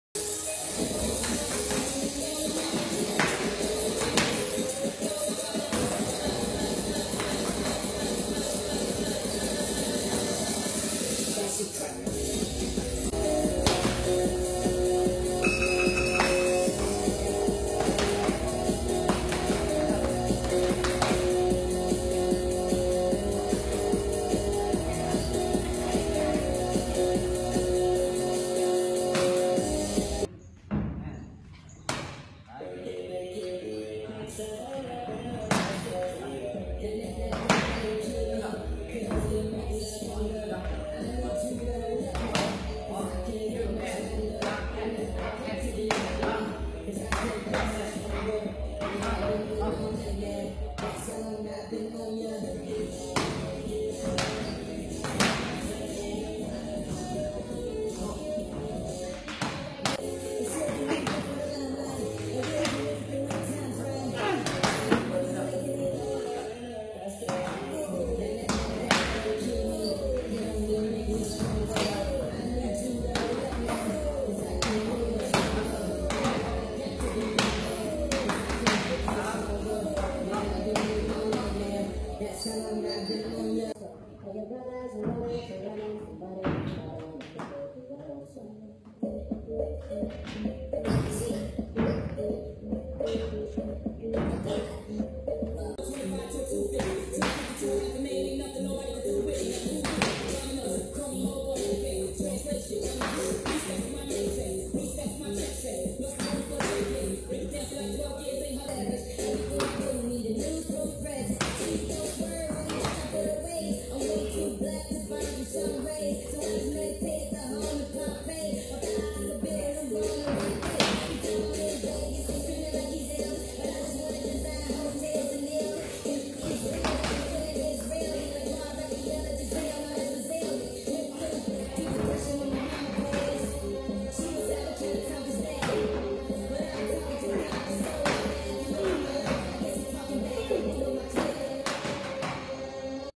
George with Begining Boxing
Carolyn Johnson on her 5th day of training. She started  Day 1 with no experience in boxing. Her progress was very fast.